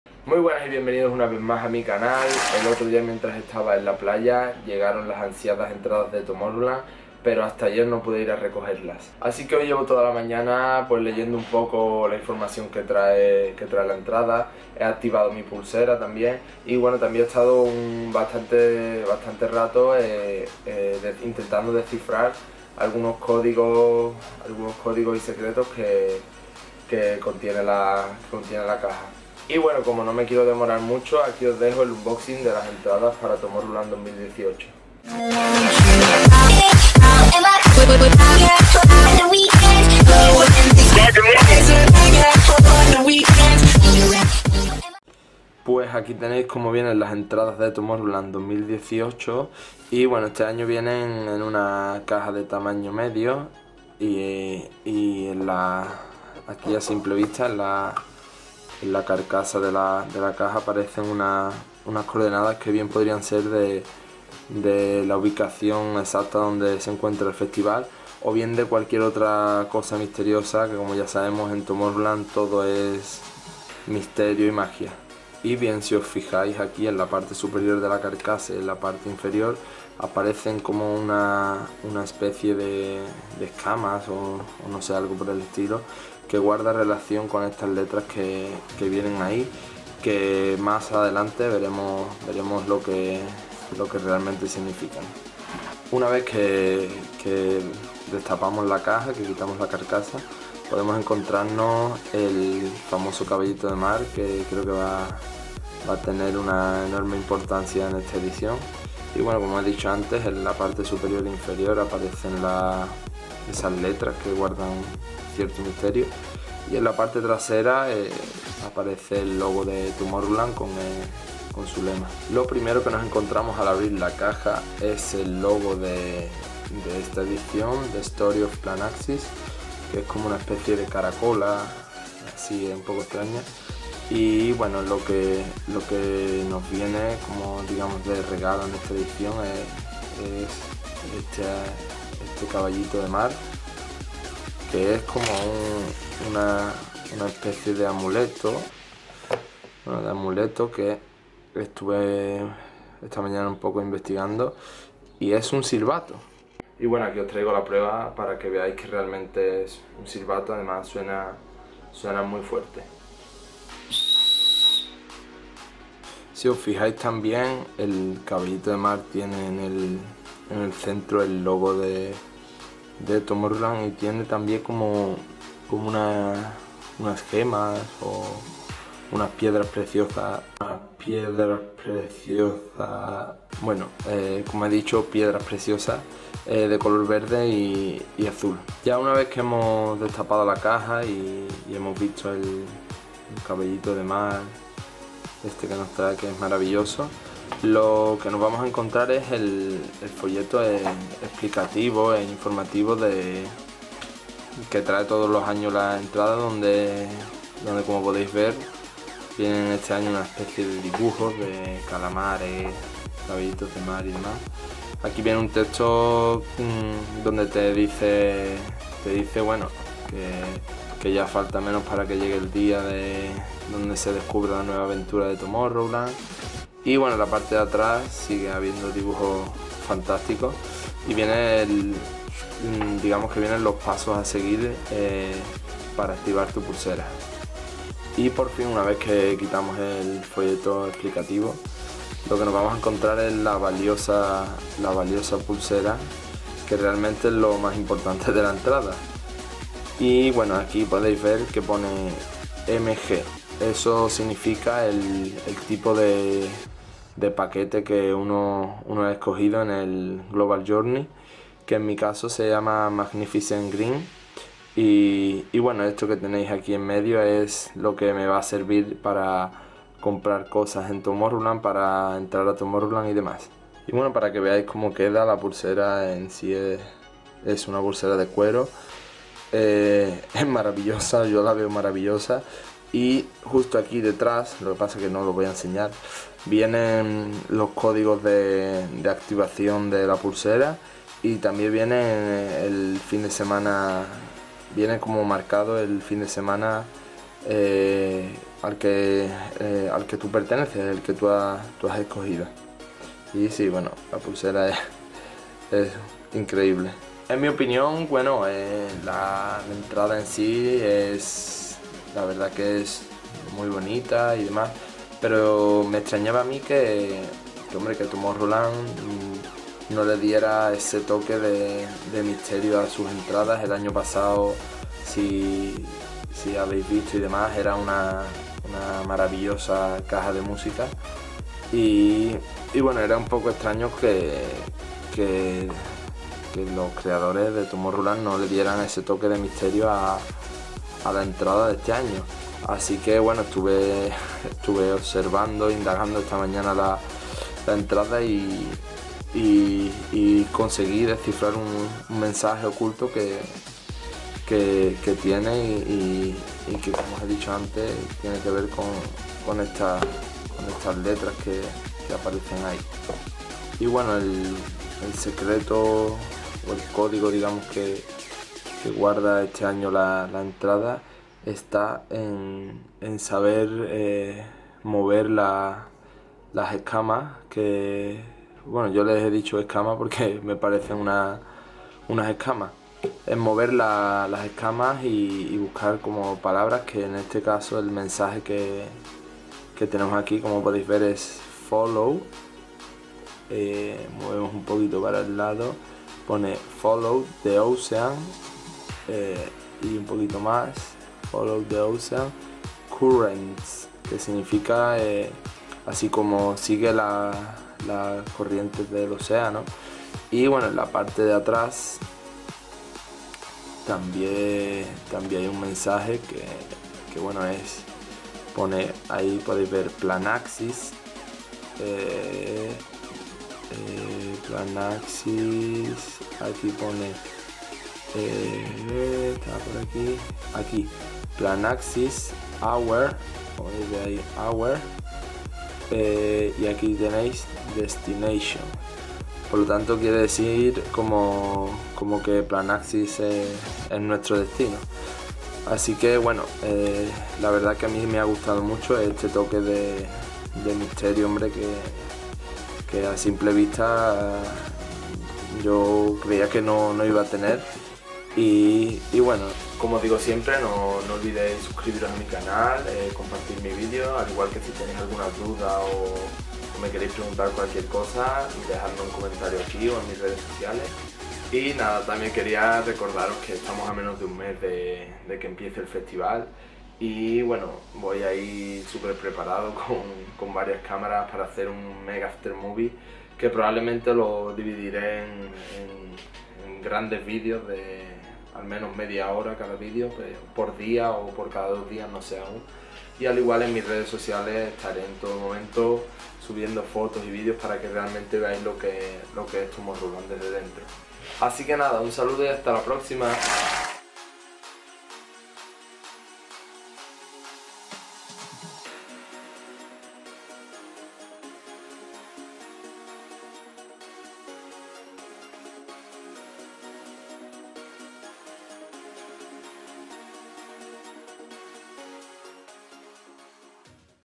UNBOXING ENTRADAS TOMORROWLAND 2018 | CHUMARI TV
En este vídeo os enseño como son las entradas de Tomorrowland 2018 (The Story of Planaxis), y os cuento también cuales son los secretos que guarda.